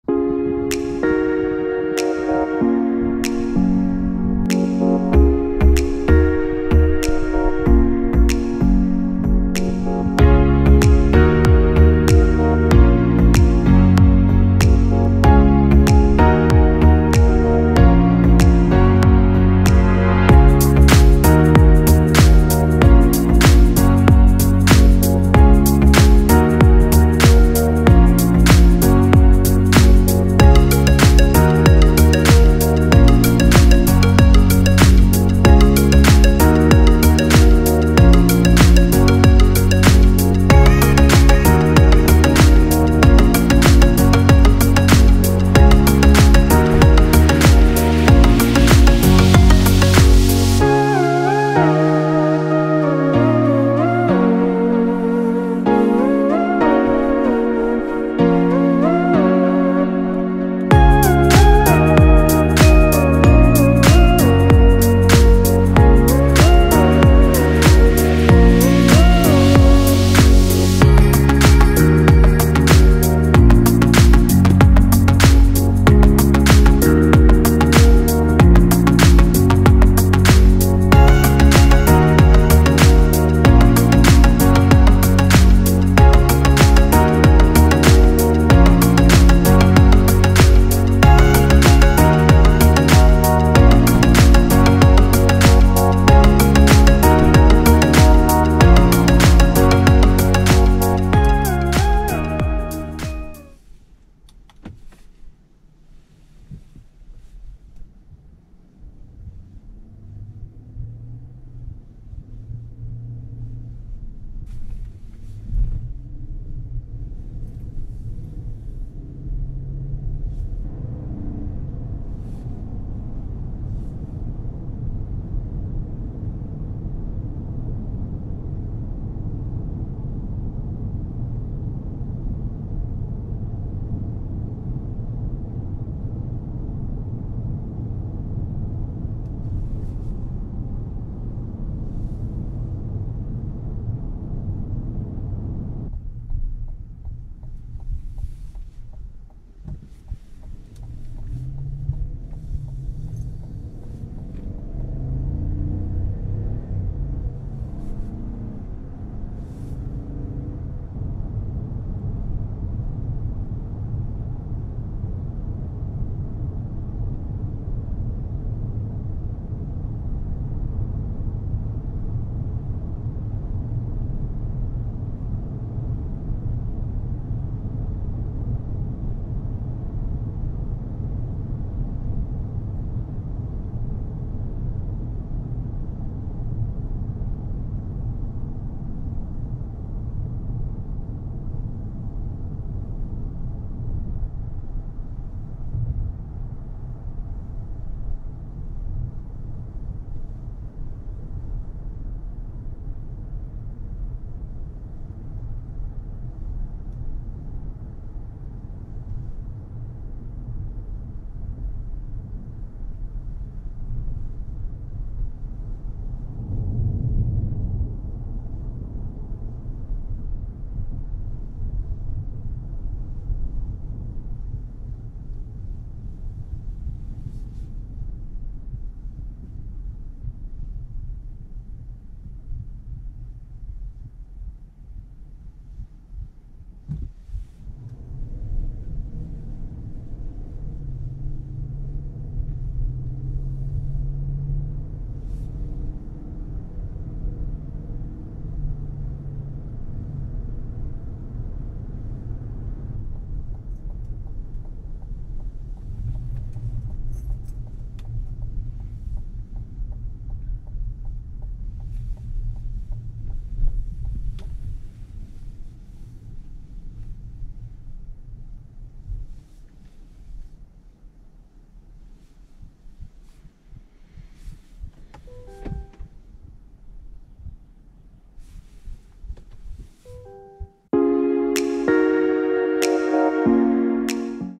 POV Drive: 2021 GMC Yukon Denali
2021 GMC Yukon 4WD Denali test drive by Car Fanatics Blog.